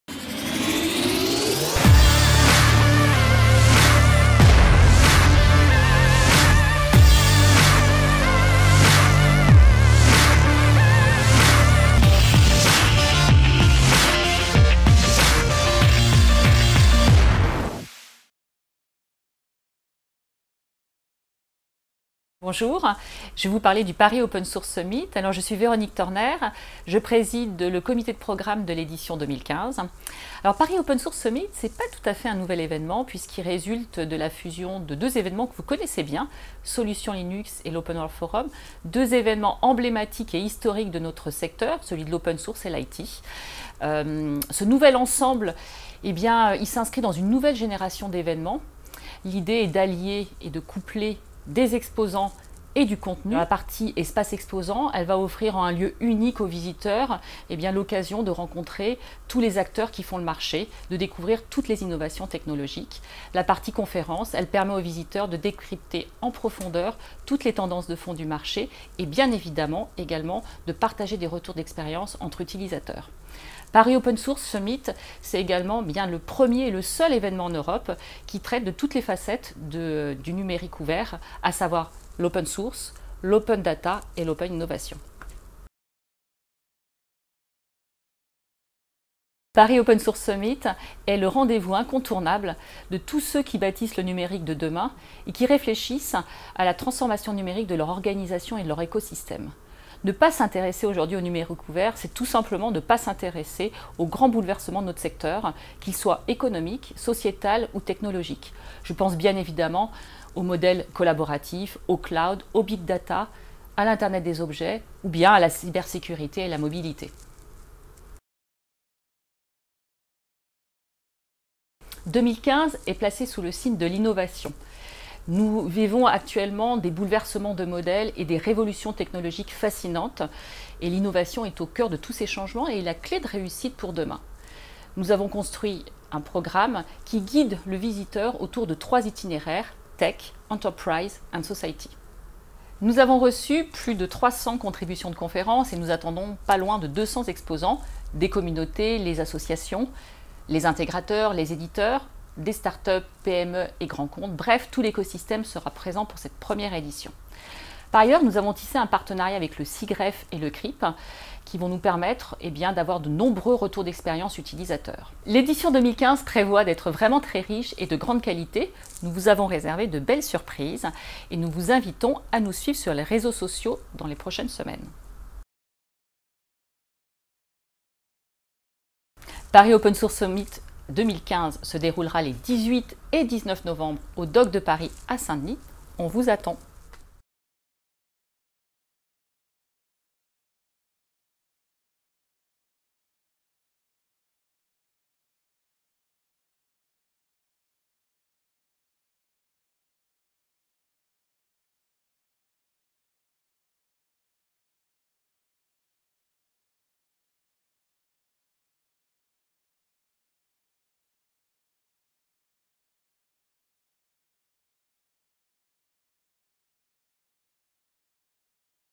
Paris Open Source Summit 2015
Paris Open Source Summit : Présentation du Premier Evénement Européen Libre & Open Source

Interview de Véronique Torner, Présidente du Comité de Programme (Présidente et co-fondatrice d'Alter Way)